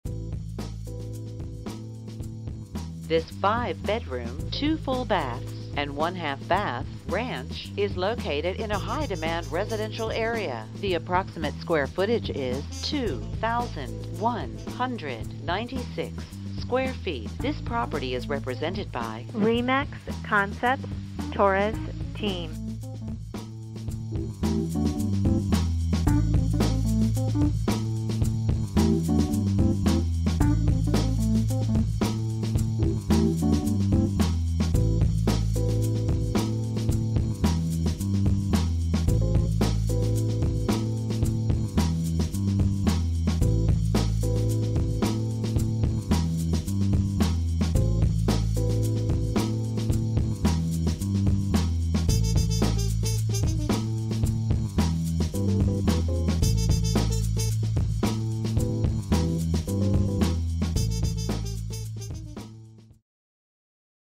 405 Woodhaven Drive, Polk City, IA, 50226 Tour - $830,000
Represented by: Torres Team
5 Bedrooms - 2 Full Baths, 1 Half Bath